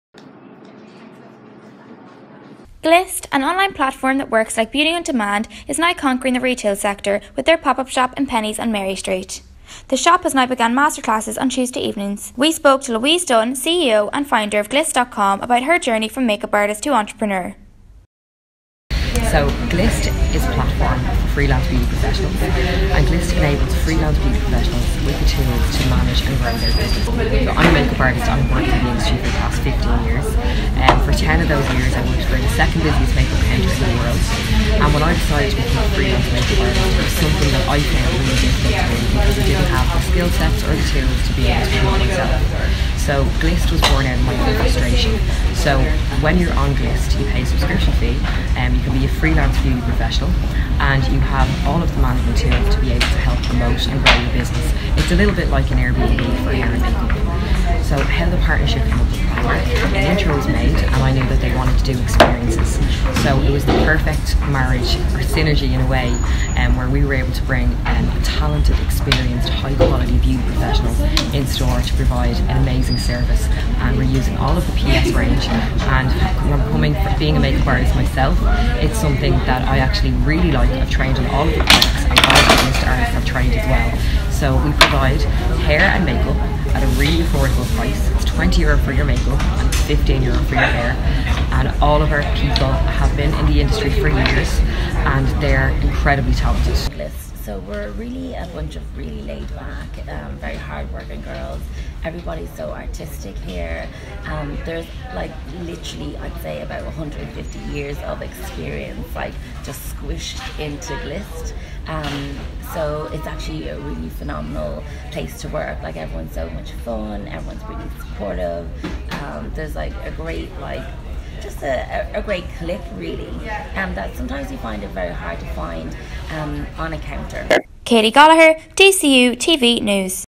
DCUtv News: Glissed video
Glissed, an online platform that works like beauty on demand are now conquering the retail beauty sector this season with their pop up shop in Penneys. Katie Gallagher reports.